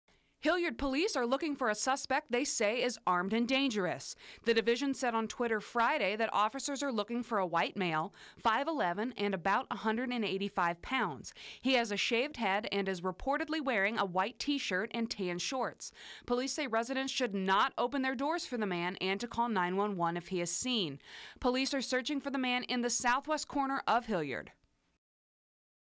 Hilliard police looking for armed and dangerous suspect
Hilliard Police are looking for a suspect they say is armed and dangerous.